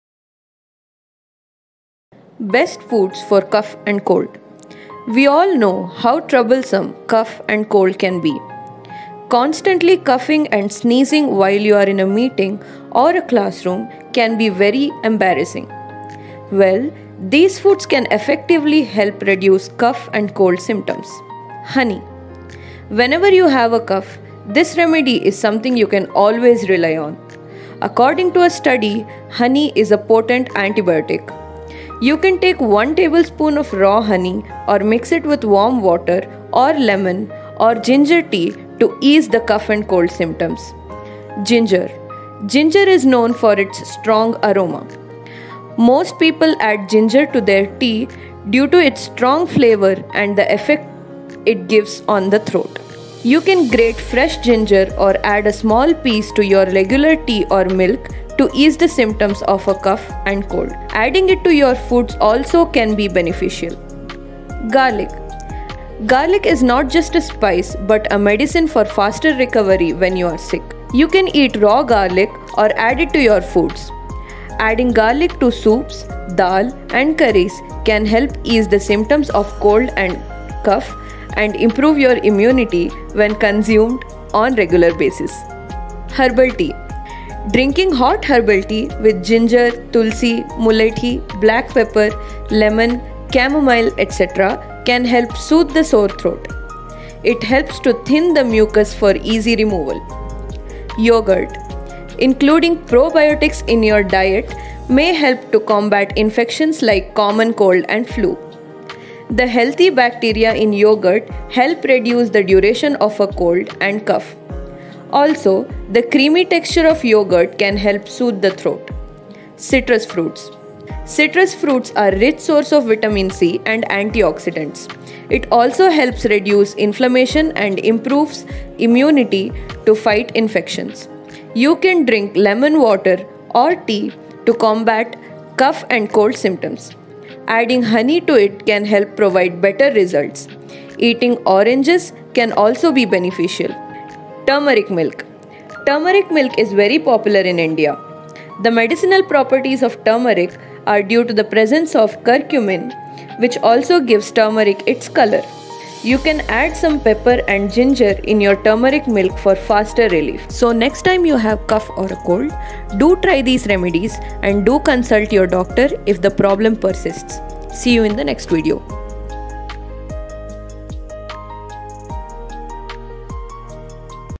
Best Foods For Cough & Cold | MedEd | English | Bibo
Hello everyone, today we will be talking about the best foods to eat when you have a cough and cold. We all know how troublesome a cough and cold can be. Constantly coughing and sneezing while you are in a meeting or a classroom can be very embarrassing. So, let's dive into the topic.

Firstly, let's understand what a cough and cold are. A cough is our body's protective mechanism. It helps remove irritants and microbes from the airways and lungs. On the other hand, a cold is a viral infection caused by different viruses. It generally lasts for a few days and resolves on its own.

So, what foods can efficiently help reduce cough and cold symptoms? Let's have a look.

Firstly, we have chicken soup. Chicken soup contains cysteine, which helps in thinning the mucus and clearing the nasal passages. Moreover, the warmth of the soup can soothe your throat and provide relief from the symptoms.

Secondly, we have garlic. Garlic contains allicin, which has antiviral and antibacterial properties. Eating garlic can help fight off the viruses and bacteria causing the cold.

Thirdly, we have ginger. Ginger has anti-inflammatory properties and can help soothe the throat and reduce coughing. It also helps boost the immune system, which can help fight off the cold.

Fourthly, we have honey. Honey is a natural cough suppressant and can soothe the throat. It also has antimicrobial properties, which can help fight off the infection.

Lastly, we have citrus fruits like oranges, lemons, and grapefruits. Citrus fruits are rich in vitamin C, which can boost the immune system and help fight off the cold. They also have anti-inflammatory properties, which can help reduce coughing and congestion.

So, these are some of the foods that can help reduce cough and cold symptoms. Make sure to include them in your diet when you have a cold.